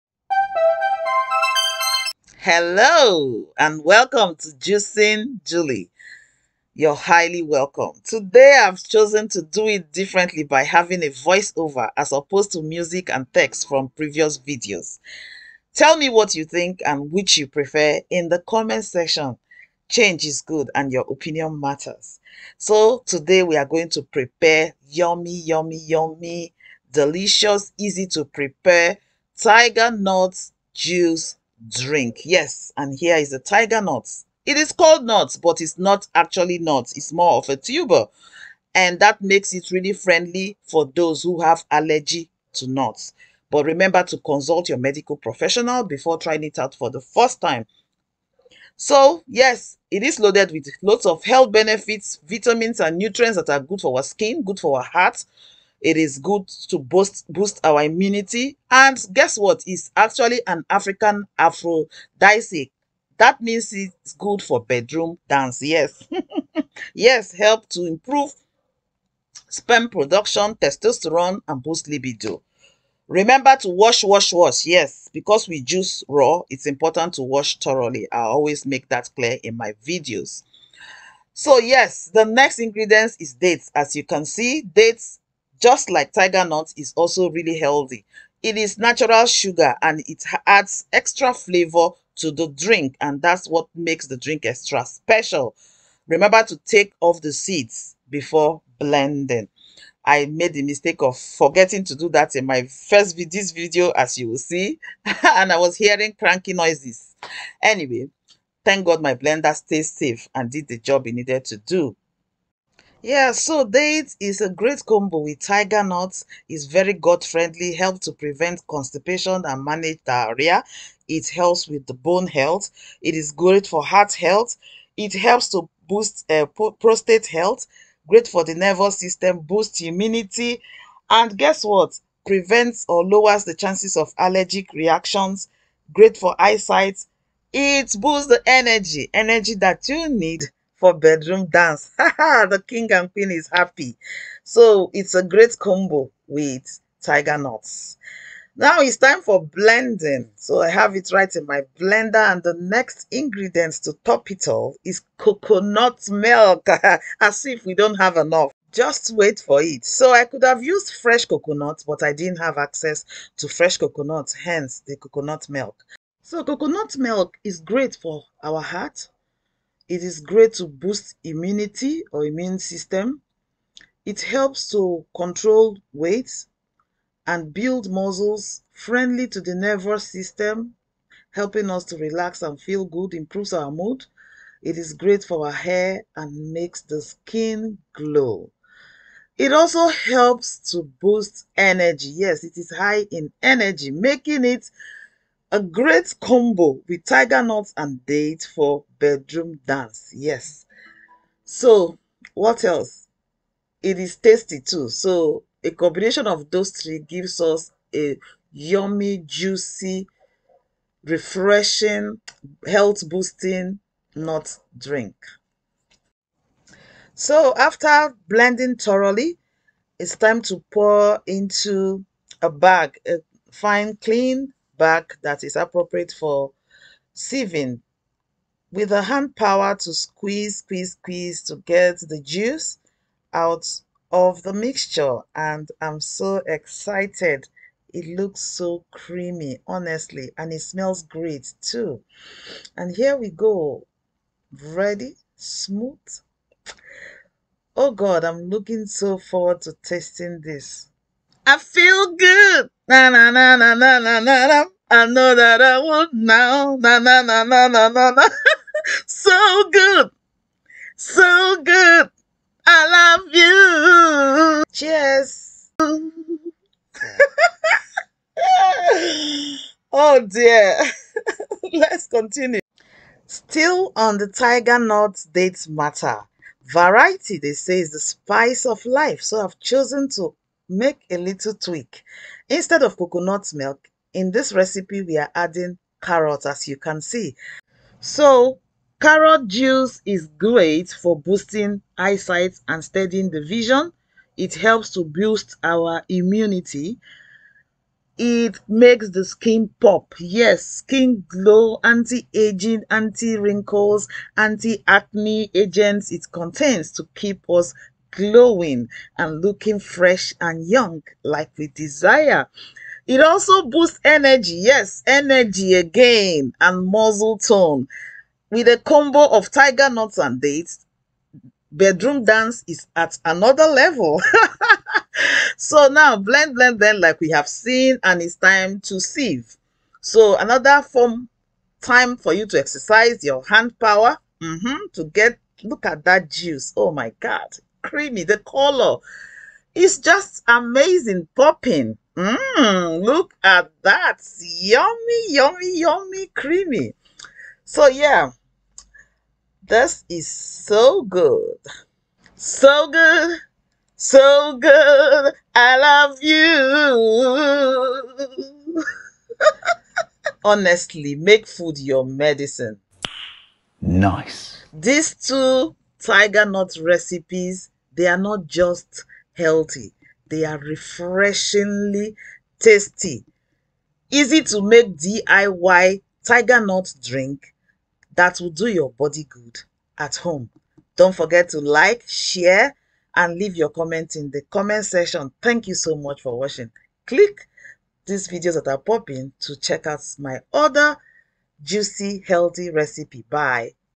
TIGERNUT JUICE DRINK IN TWO TASTY RECIPES // BEST AFRICAN APHRODISIAC DRINK
The most delicious, healthy, easy to make tiger nut juice drink made in two creamy recipes. No added sugar. All natural ingredients. Tiger nut, dates, coconut milk, carrots combo juice drink. Contains loads of health benefits and its refreshing too.
It is also a known African aphrodisiac as it boost energy, improves prostate health, great for long lasting stamina, fertility, reproduction, pregnancy and nursing mothers.
These two recipes are delicious, easy to make and refreshing too. Click on the video link to WATCH!